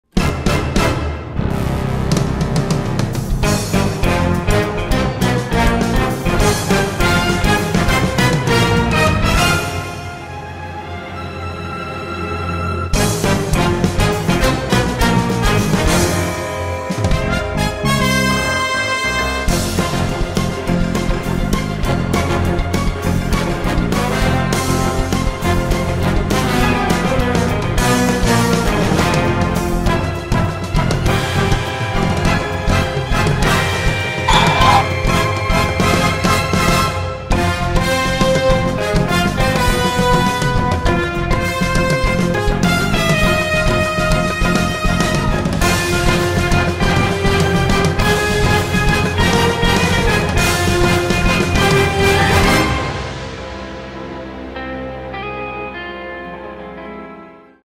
Bande annonce USBDA 61 - BCMF
1er match de l'histoire de l'USBDA 61 en LF2. C'est maintenant !